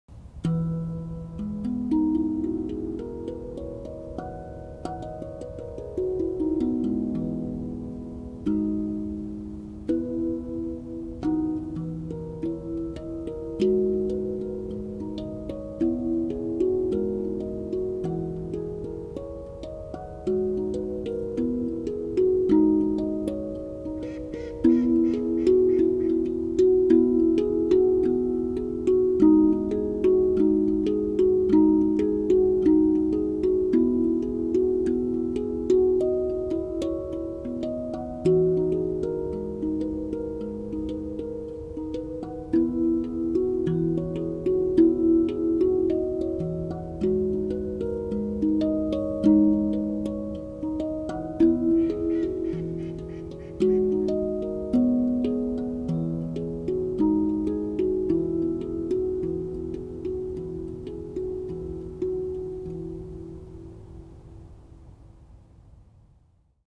Orion drum, medium size, E golden arcadia 12 scale
A new model in medium size.

Diameter 39CM / height 15CM / weight 3.5KG